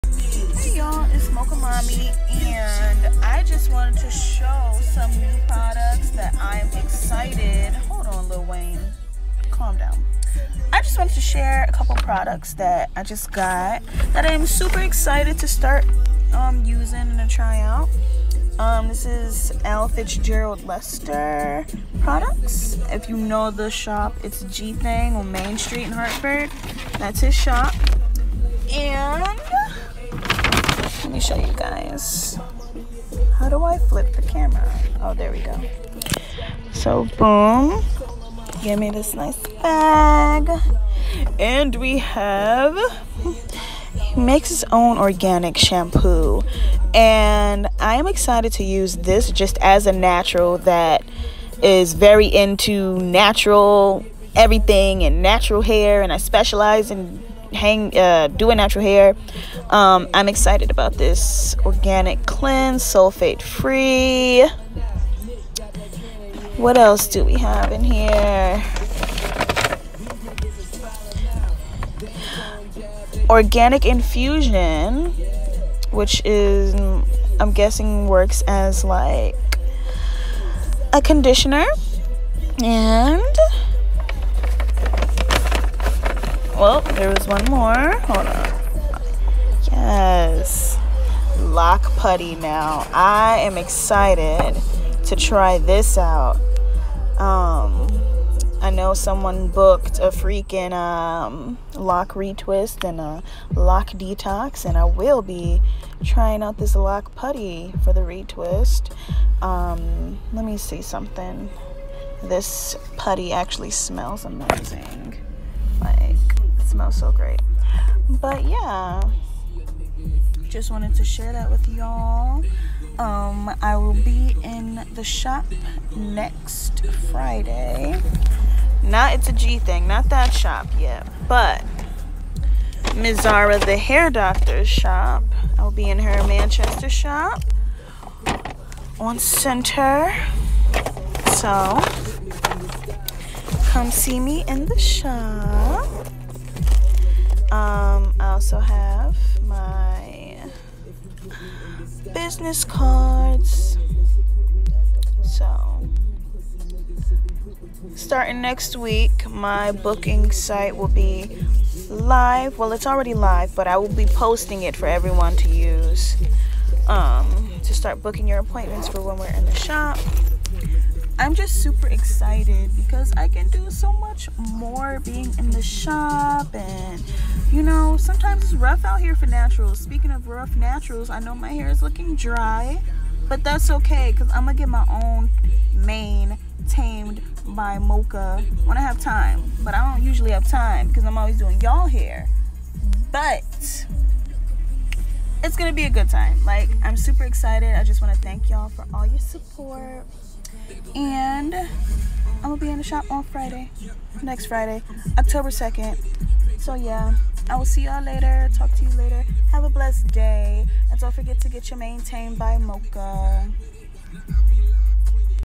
New Products!
Mocha Mamí sharing some new products that she was given✨